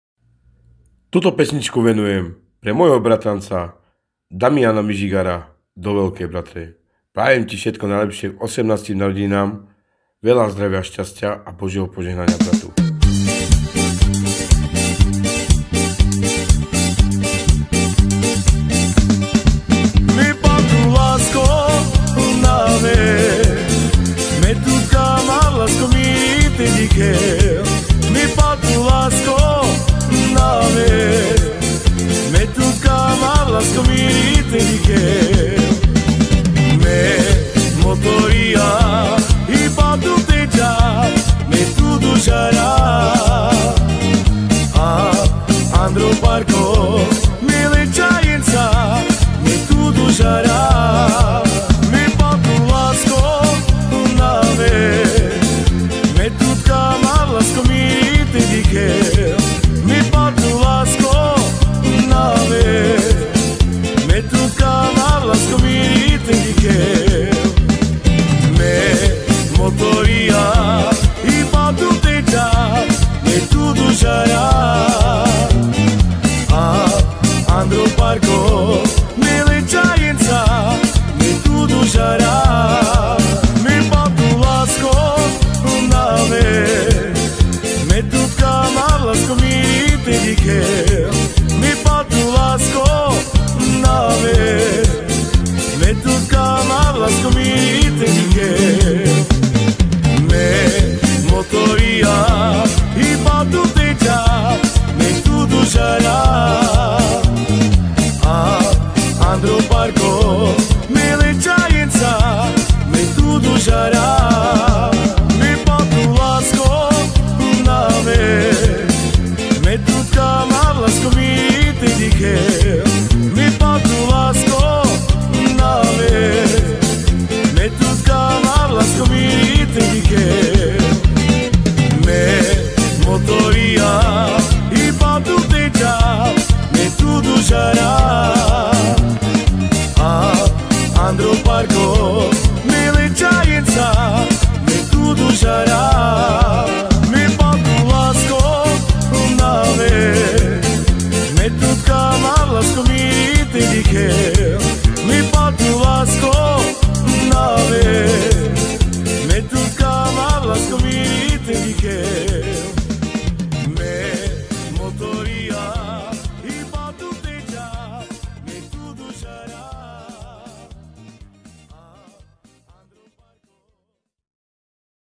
Kariško official venujem pesnicku pre môjho bratranca diamant mižigara k 18 narodeninám 🥳
Kariško official